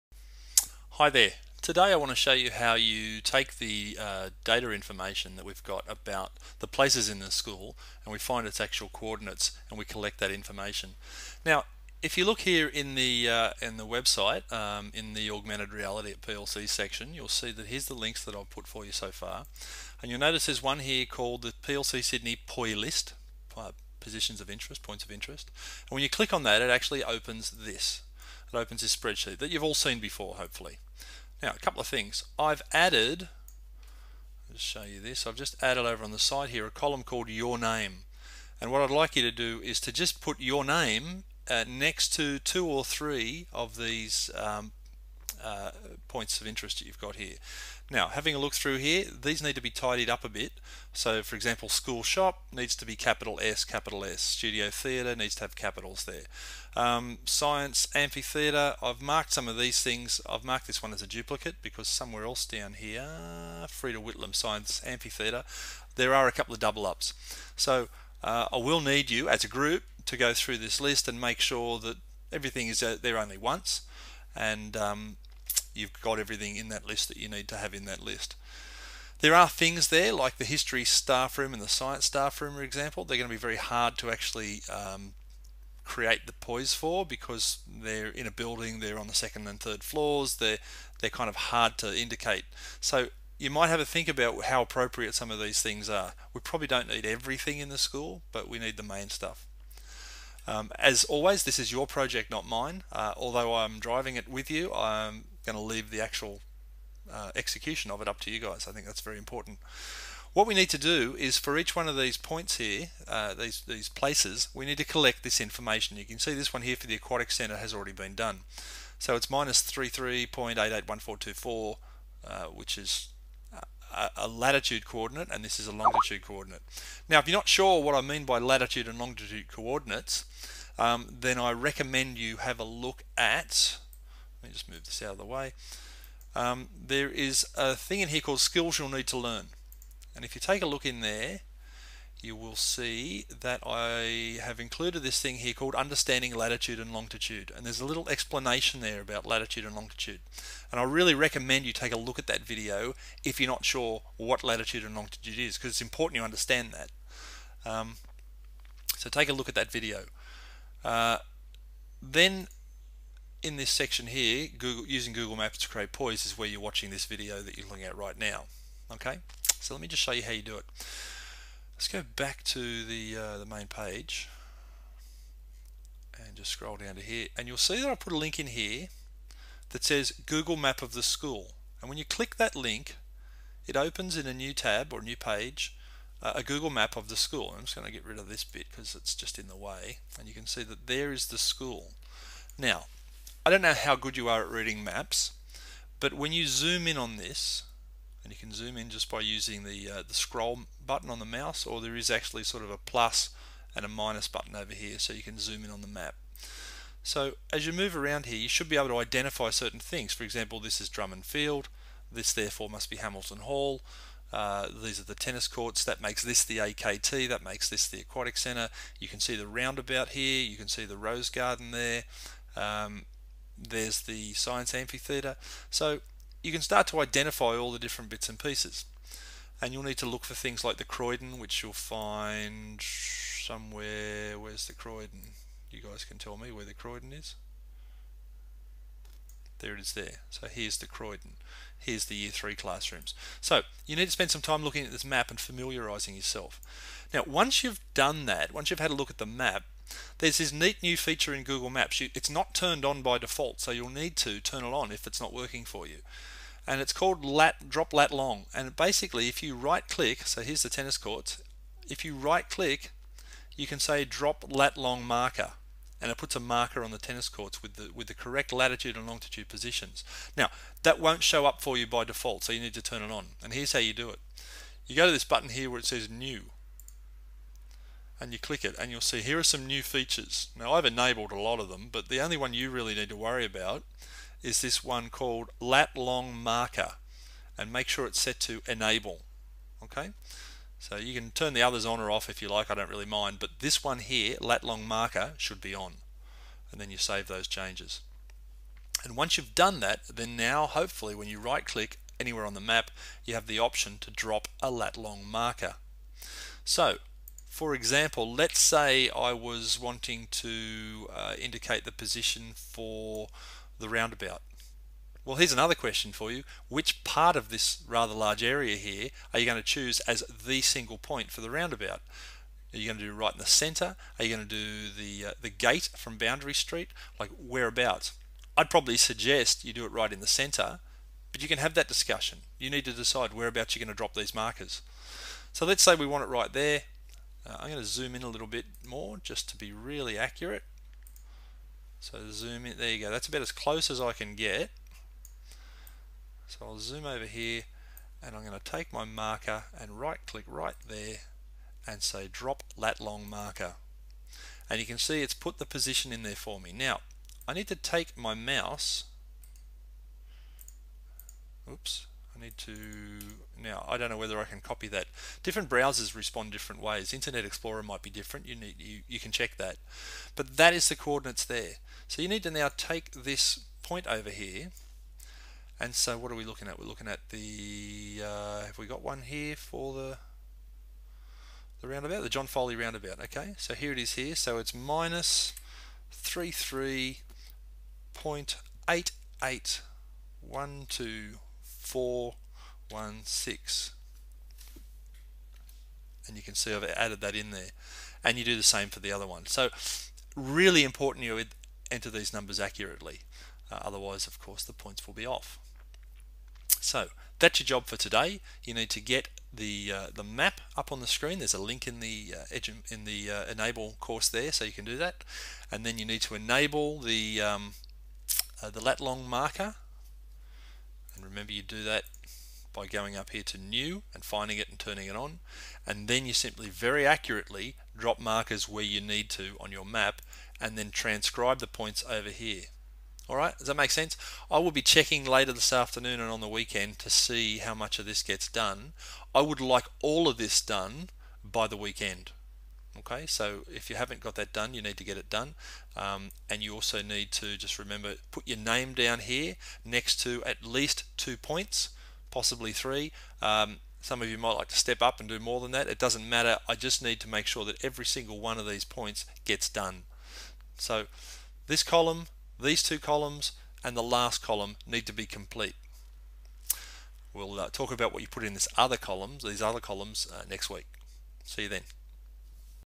LatLongMarkers.mov
A detailed description of how to add the Latitude and Longitude markers to the Google Spreadsheet we are using to collect our POI information.  Explains how to enable LatLngMarker feature and how to get the information into the sheet.